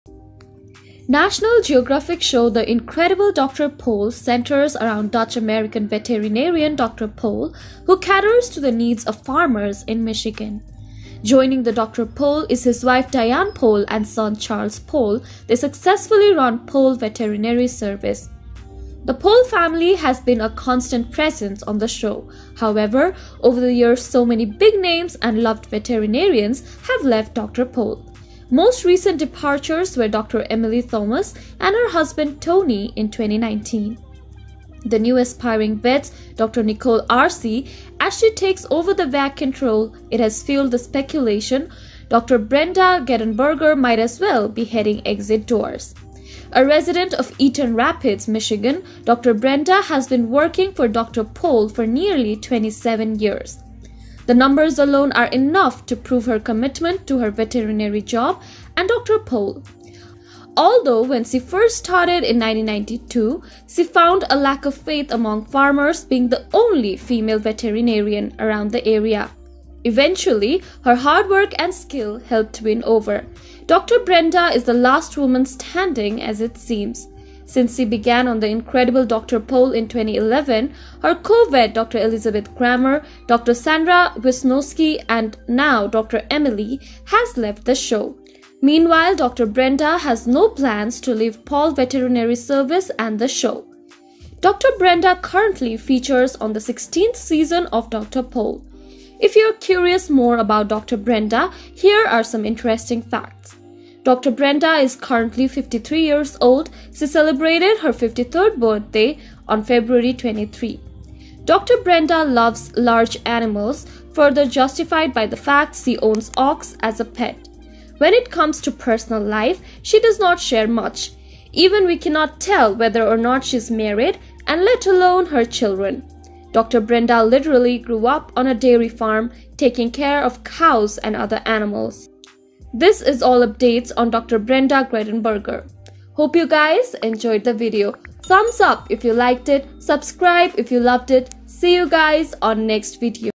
Is Dr. Brenda Grettenberger Leaving " The Incredible Dr. Pol"..?
What happened to dr Brenda Grettenberger on " The Incredible Dr. Pol"..? is dr Brenda Grettenberger leaving “The Incredible Dr. Pol"..? What Dr. Brenda Grettenberger doing now in 2020? Dr Brenda facts. watch this video to know more abour Dr. Brenda.